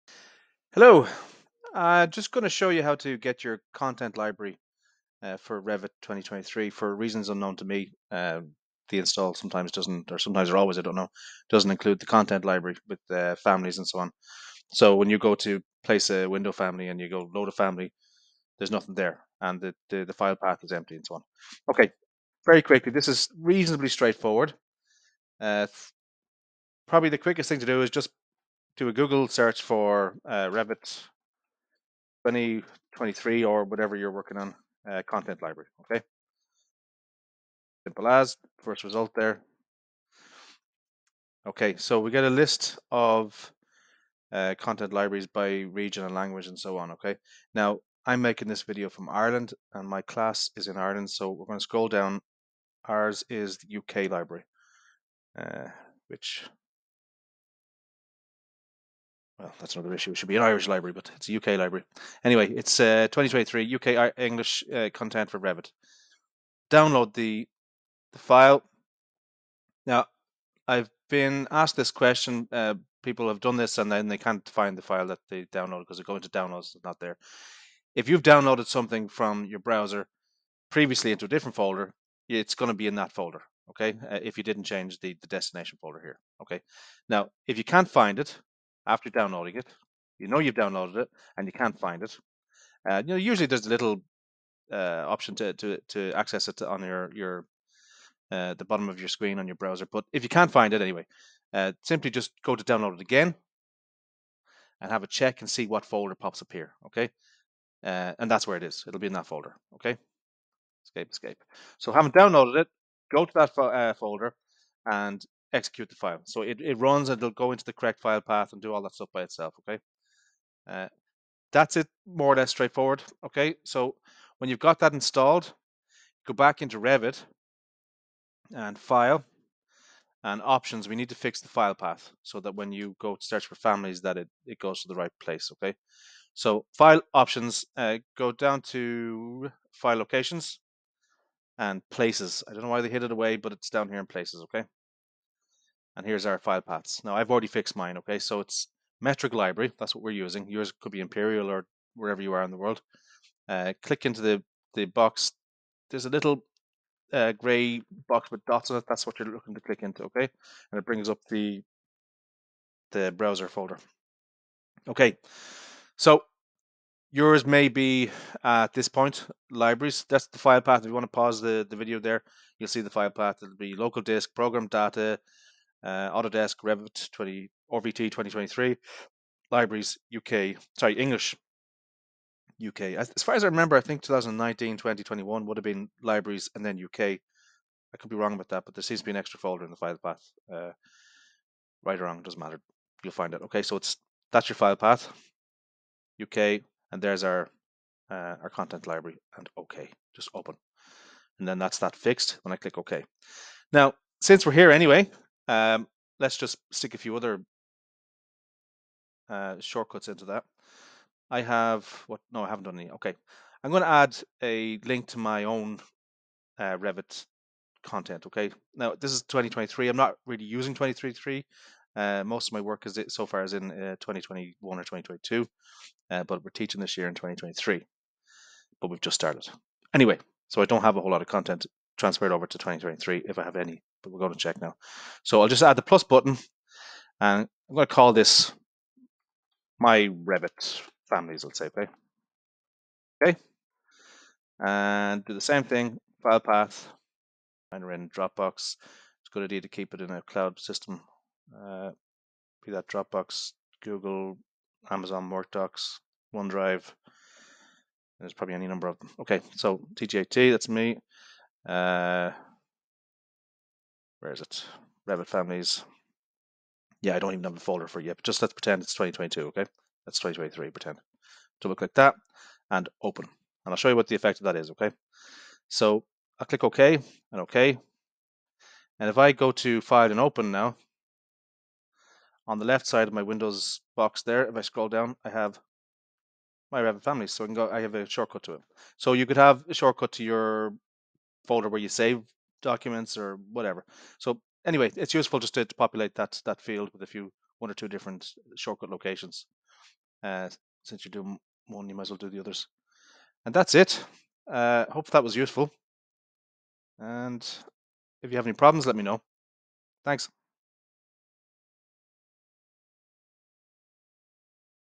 Find and Install Revit Content Library
Download and install content library for Revit. Fix the file path to the family library. Add shortcuts to your own Revit content.